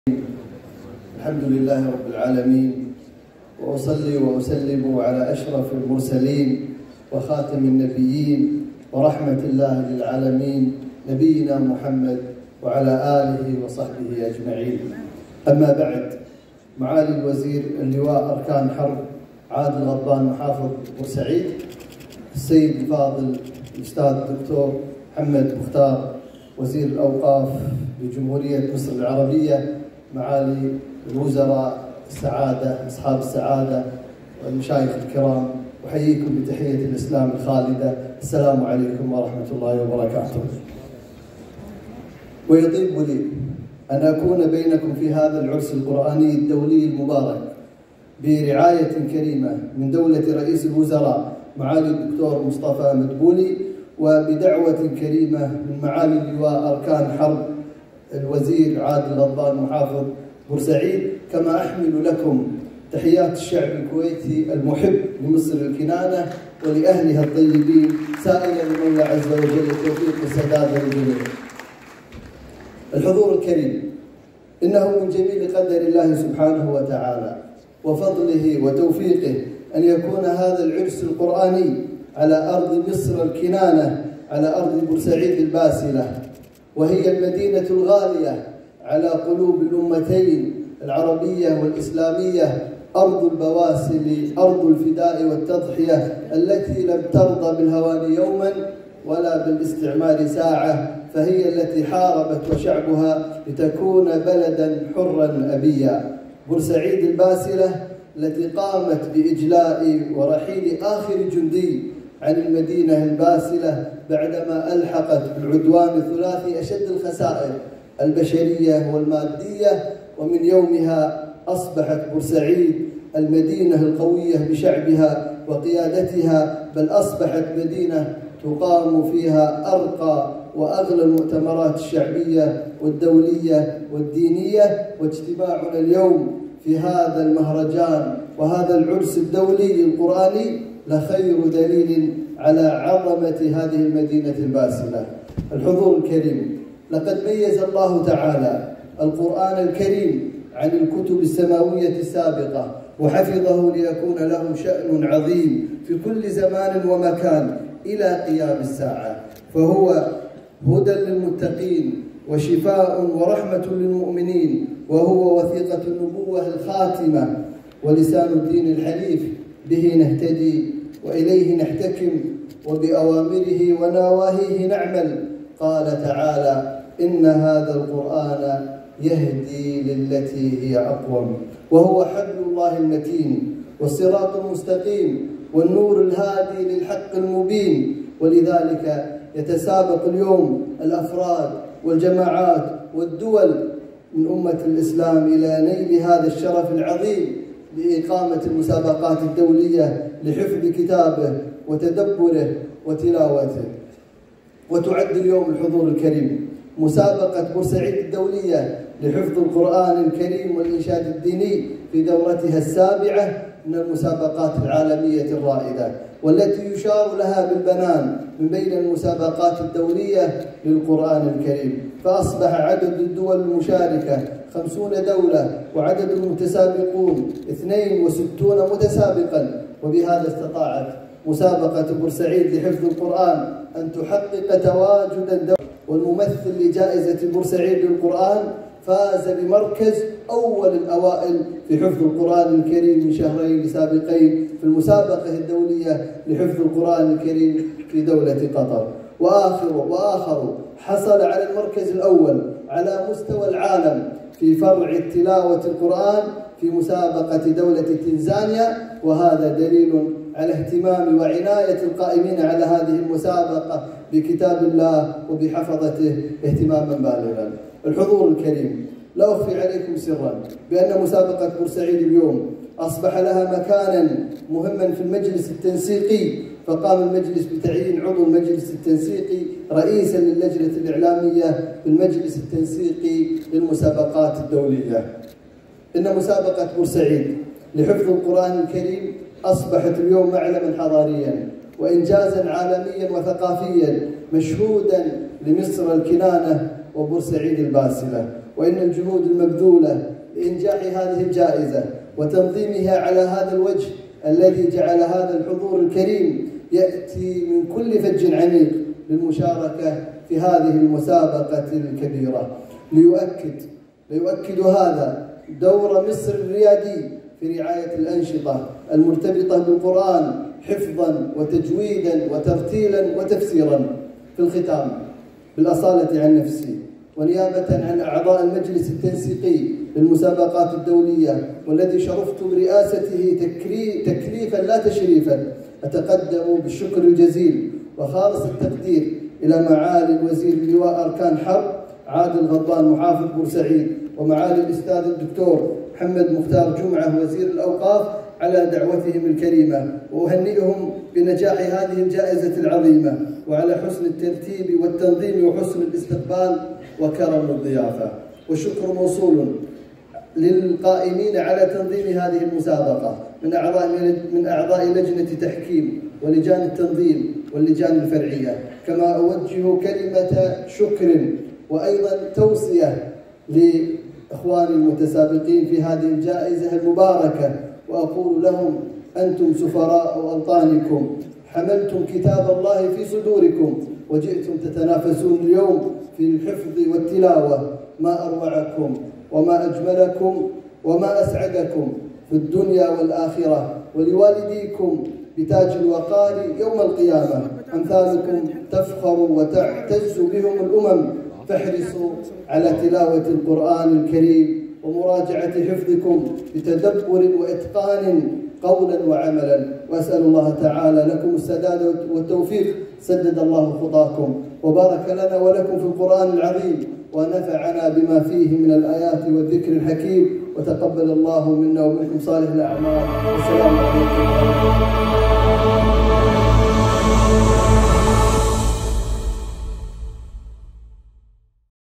رئيس المجلس التنسيقي للمسابقات القرآنية الدولية : مسابقة بورسعيد أصبحت من أهم المسابقات
ما عليك سوى الانتساب إلى هذه القناة للاستفادة من المزايا:
ElBaladOfficial#
ElBalad_Official#